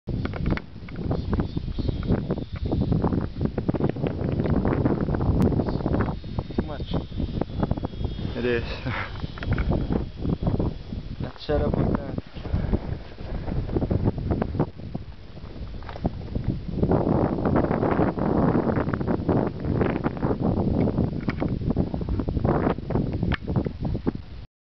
On the great wall
Standing on the great wall in China, not really the tourist area. on average, it takes about 5 hours to walk this 10km route... it was simply amazing.